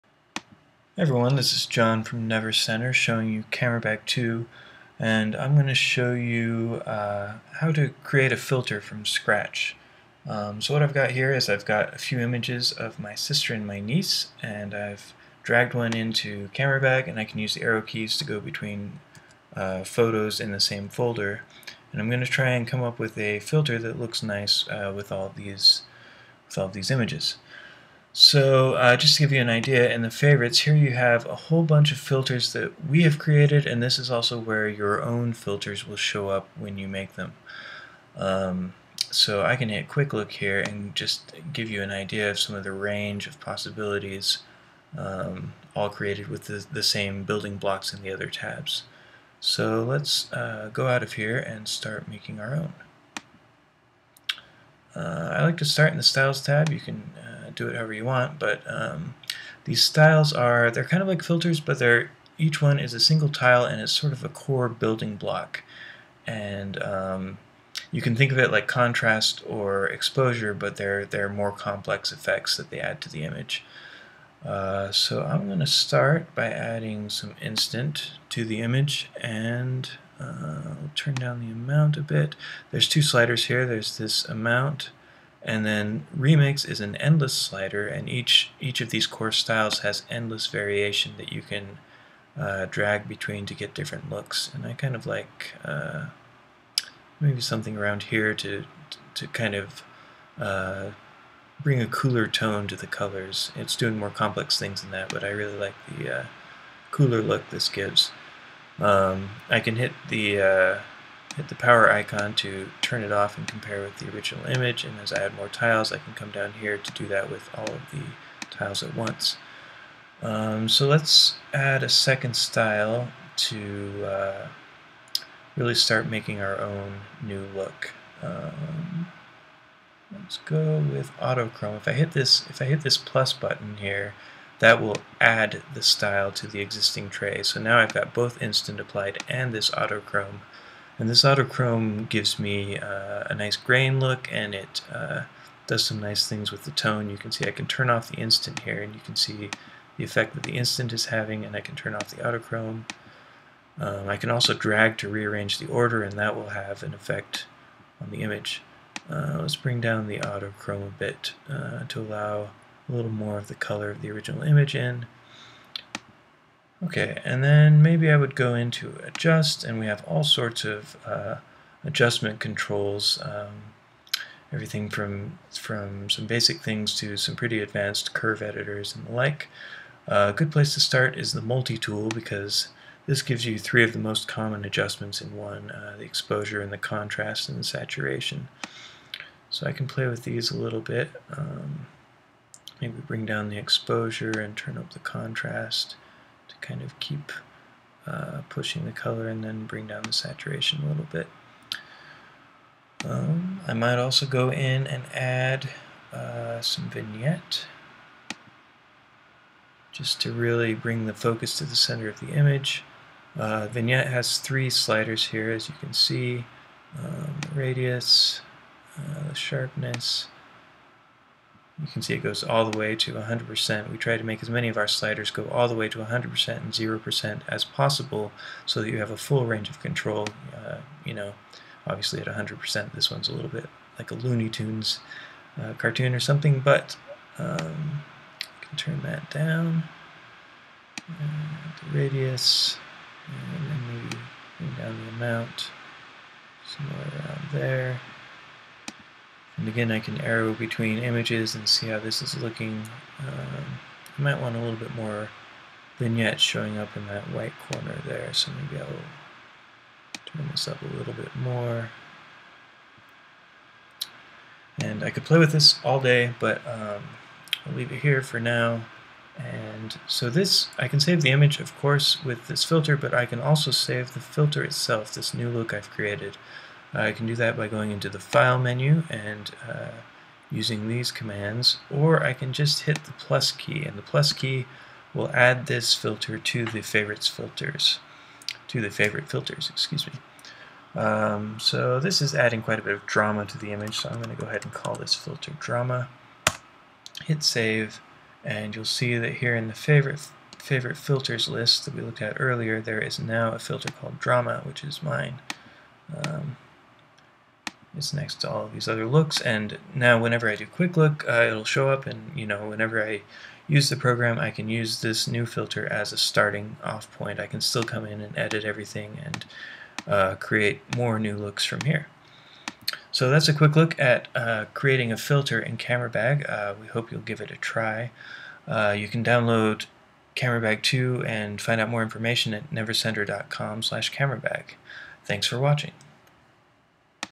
CameraBag 2 Photo Editing: Creating A New Filter (Drama)
An example of how to create your own photo filters in CameraBag 2 for Mac and PC. Find out more about CameraBag 2 and download a free demo or via the Mac App Store for the OSX-only version.